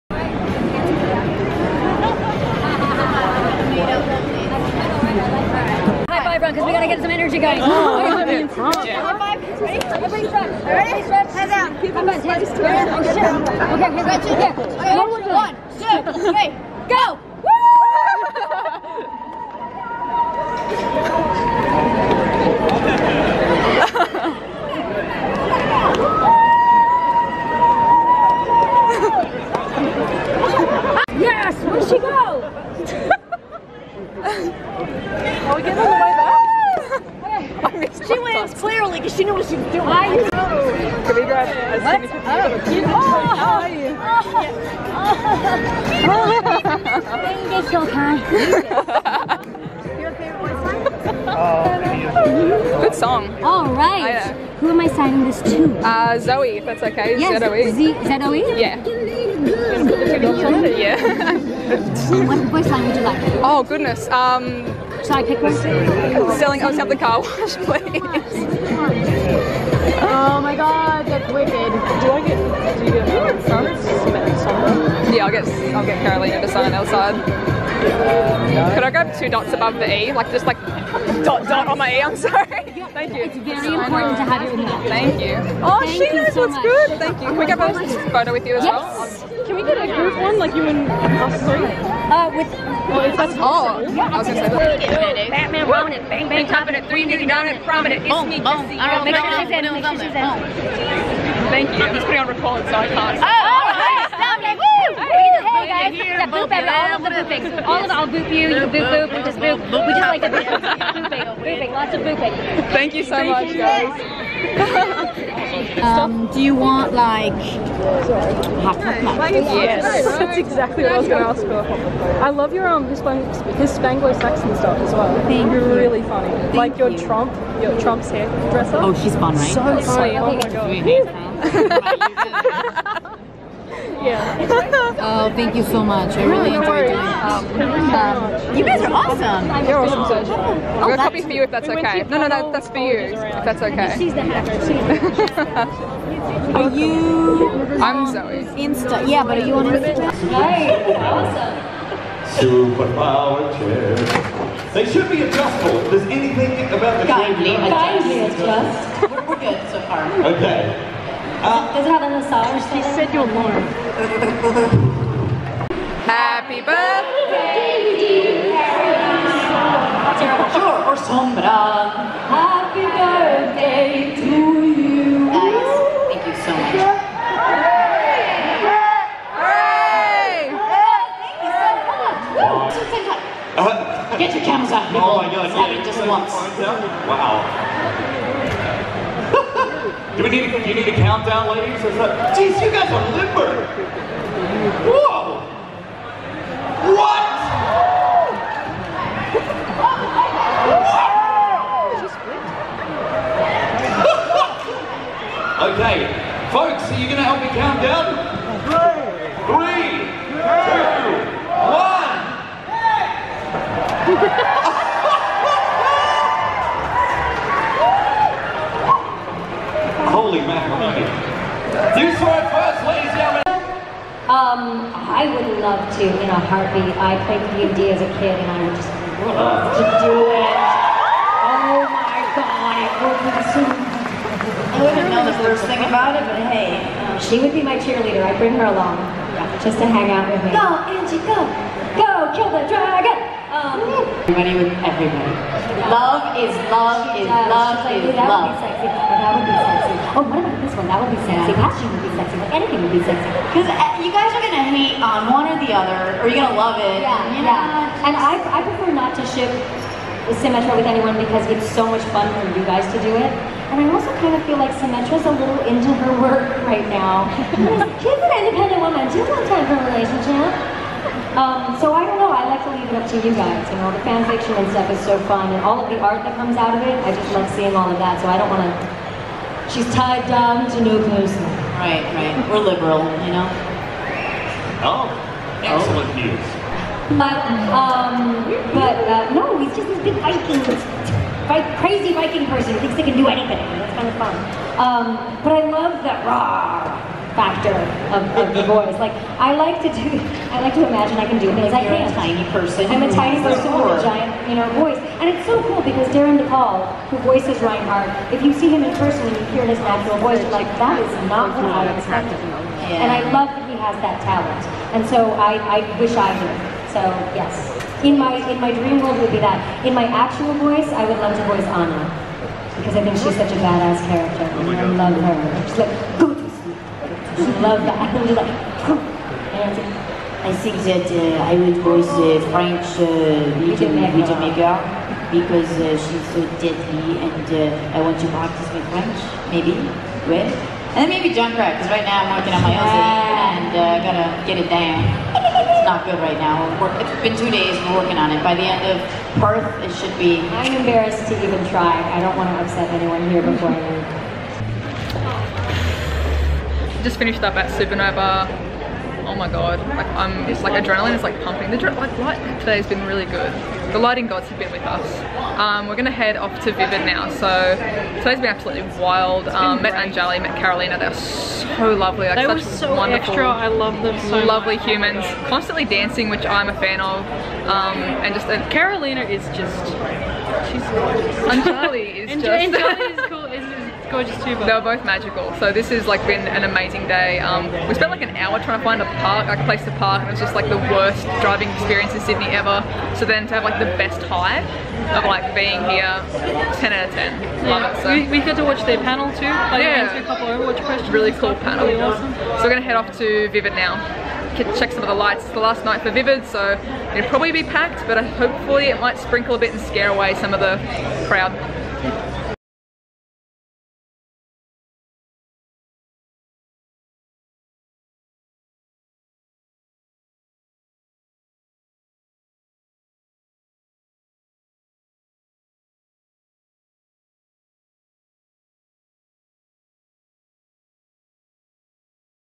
SUPANOVA SYDNEY 2017 | Meeting Anjali Bhimani & Carolina Ravassa Overwatch Voice Actors | ZoëTwoDots
Had an absolute blast meeting these two and watching their Q&A panel. We met Overwatch voice actors Anjali Bhimani (Symmetra) and Carolina Ravassa (Sombra) at this years Sydney Supanova 2017!

GEAR:
Camera: Canon G7X Mark II
Tripod: JOBY Gorillapod
Editing: Adobe Premiere Pro